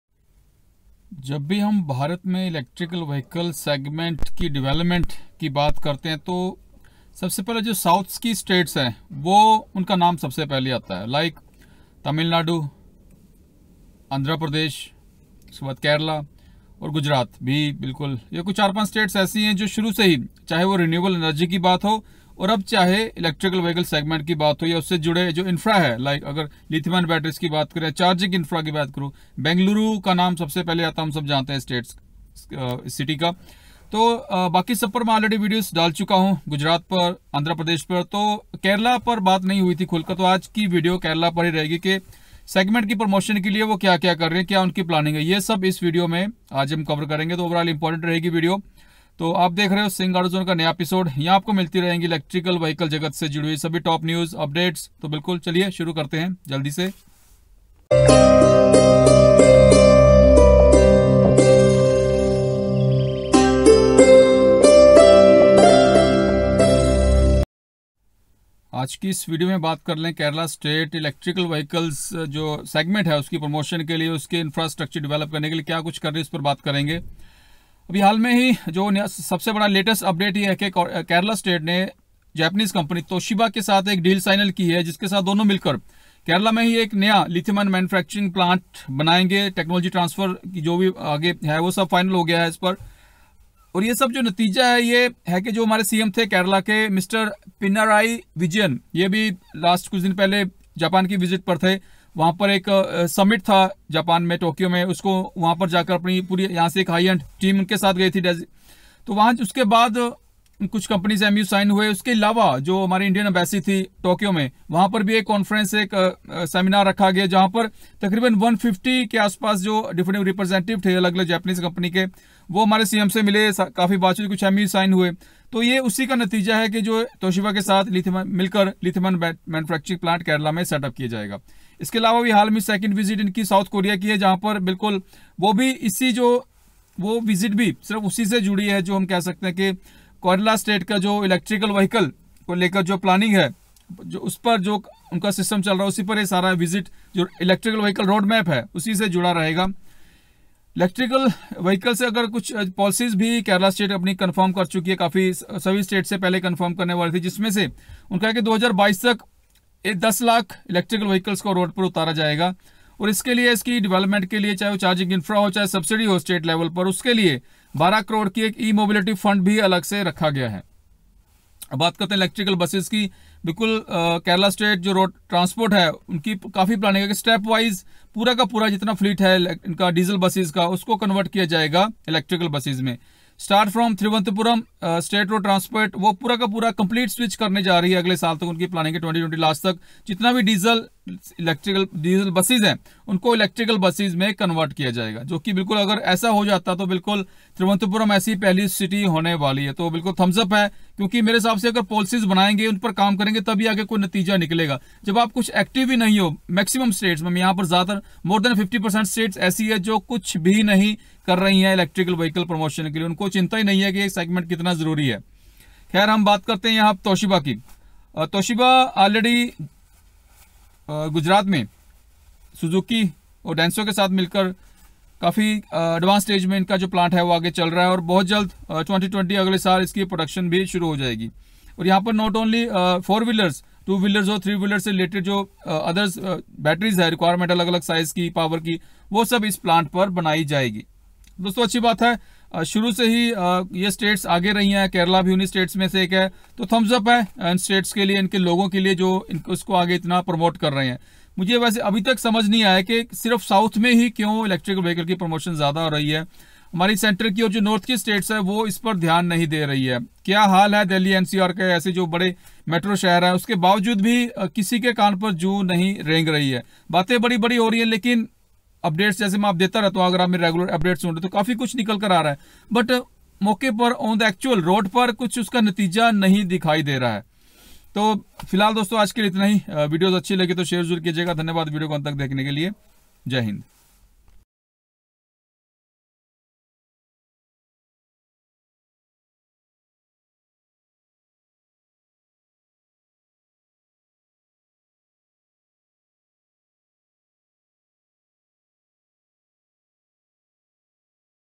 Electric vehicle segment development in India//EV infrastructure//EV news//Toshiba li battery plant.
Welcome to SINGH AUTO ZONE, in this video i share Electric vehicle segment development in India is mainly in sothern states. In this video i share kerla state planning and policies on ev infra and development. Main update is kerla state deal with japanese company Toshiba to set up new lithium ion battery plant in kerala. Also plan to add electric buses in their fleet.